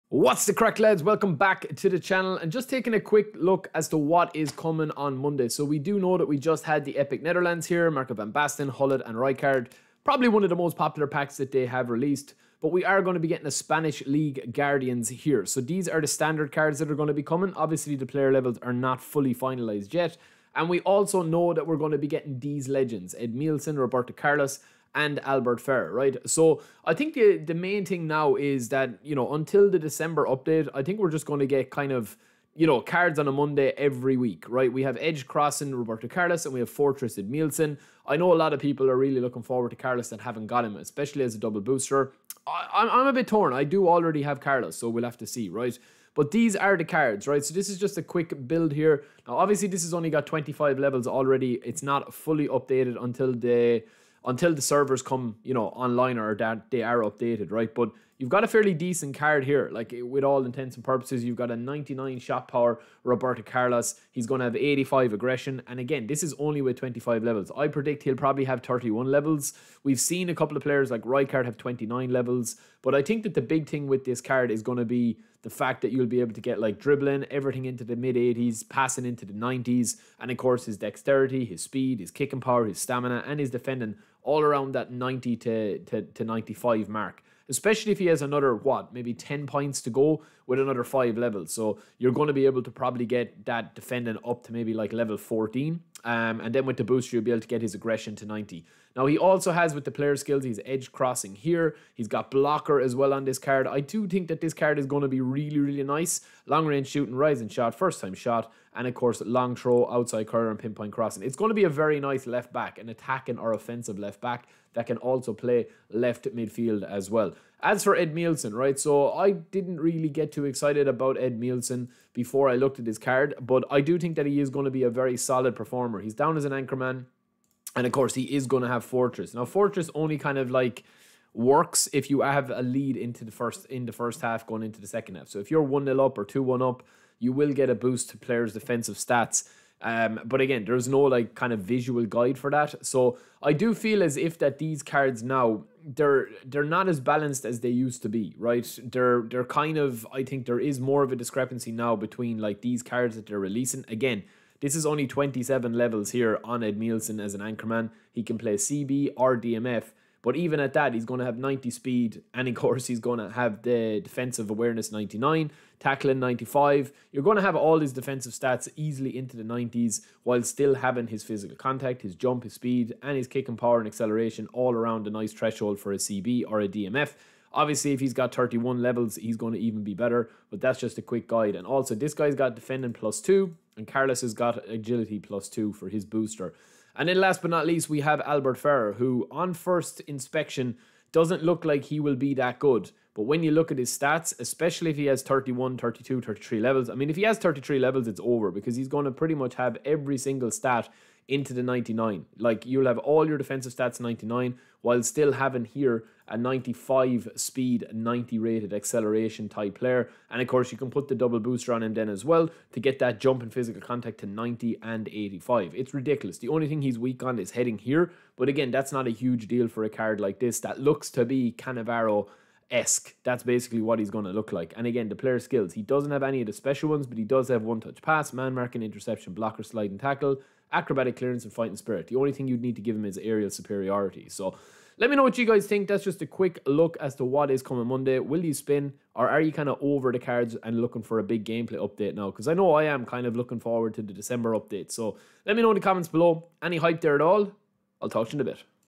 What's NEW Monday | Carlos, Fortress Edmilson & All 99 Ferrer
Mondays pack sees the release of Spanish guardians and also the return of Carlos edged crossing, Ferrer who goes to all 99 in defensive stats and fortress edmilson